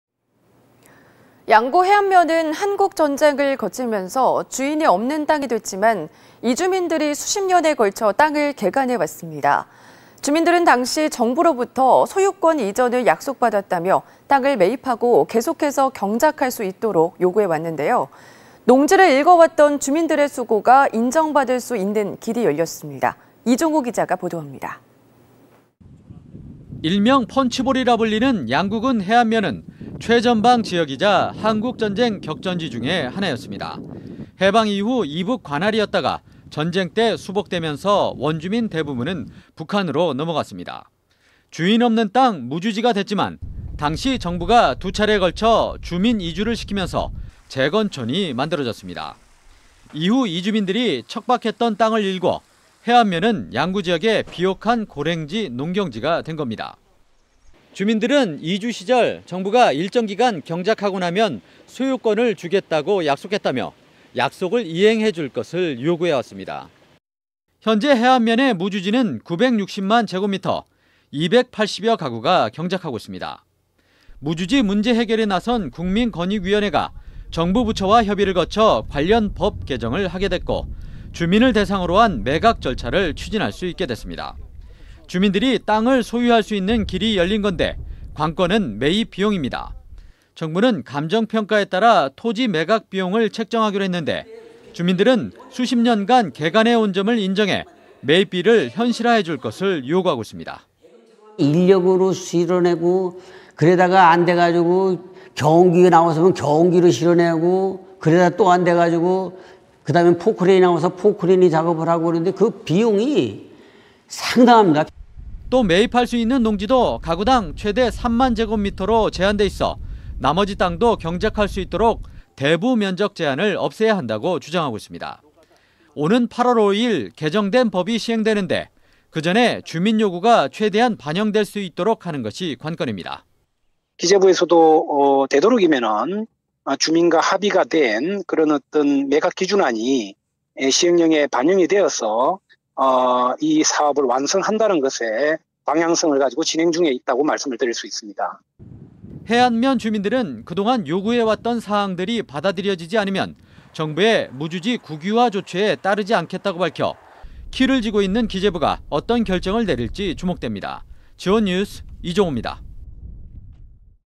[G1뉴스] 양구 해안면 '수십년 개간' 인정받나
양구 해안면은 한국전쟁을 거치면서 주인이 없는 땅이 됐지만, 이주민들이 수십년에 걸쳐 땅을 개간해 왔습니다.

 주민들은 당시 정부로부터 소유권 이전을 약속 받았다며, 땅을 매입하고 계속해서 경작할 수 있도록 요구해 왔는데요,

 농지를 일궈 왔던 주민들의 수고가 인정 받을 수 있는 길이 열렸습니다.